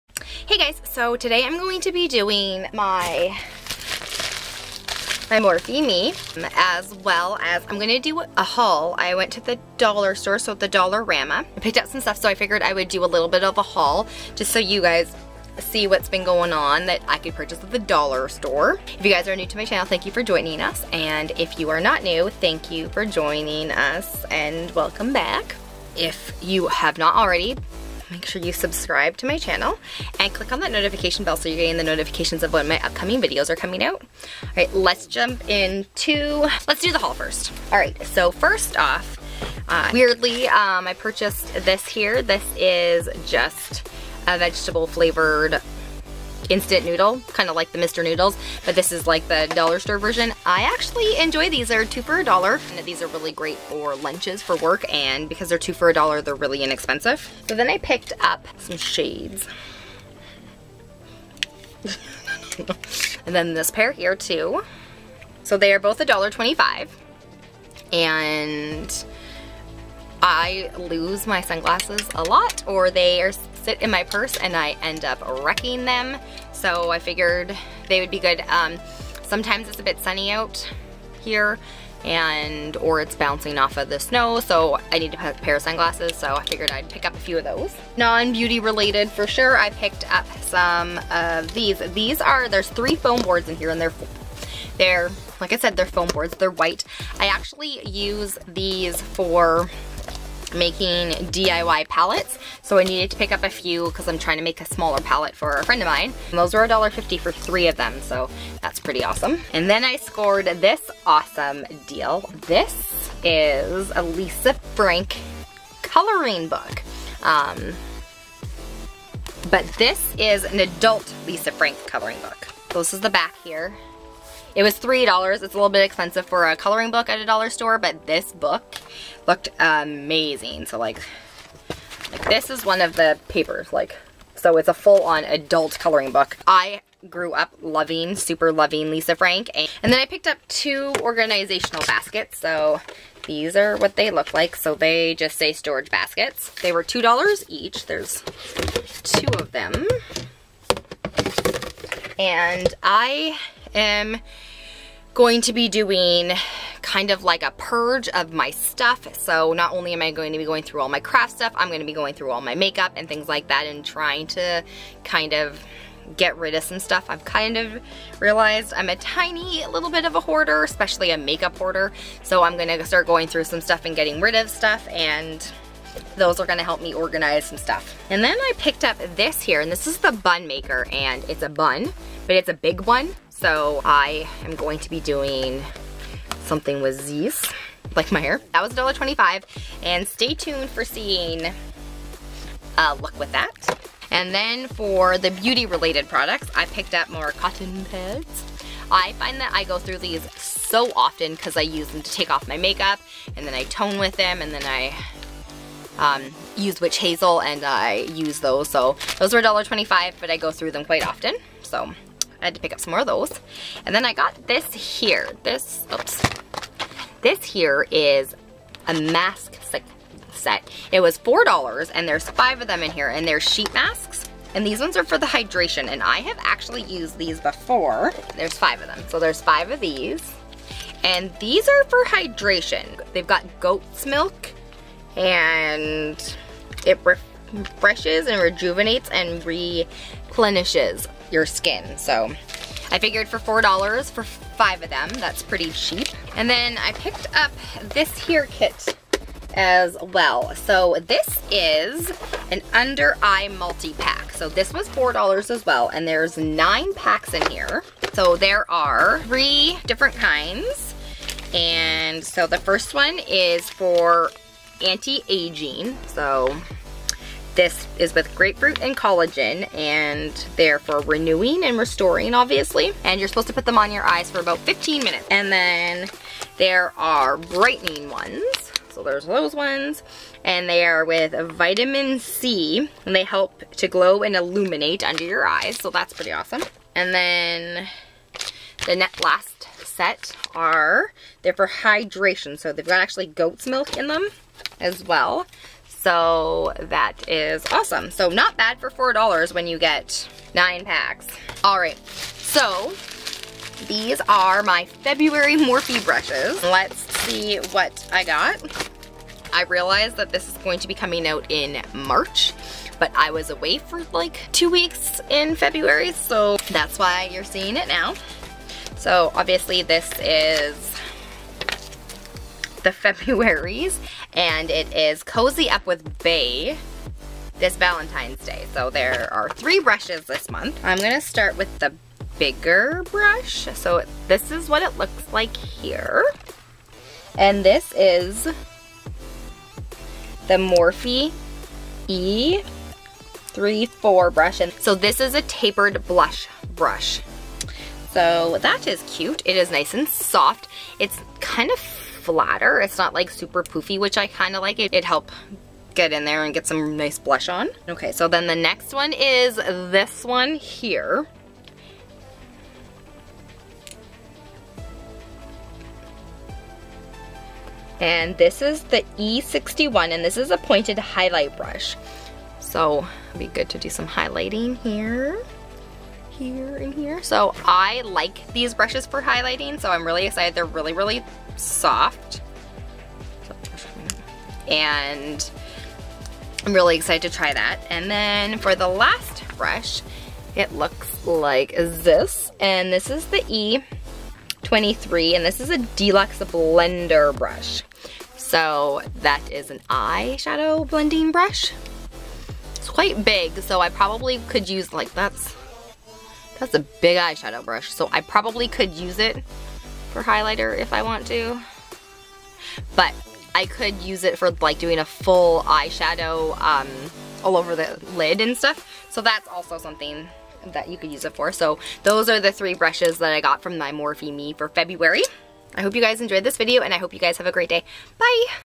Dollarama haul and feb morpheme
Hey guys so here is a dollarama haul  and my February morpheme unboxing. sorry the unboxing is so late. if you want to skip the haul and jump to the morphe brushes head to 5:00 to see the unboxing.

If you would like to purchase your own morpheme  brush subscription you can head over to liveglam.com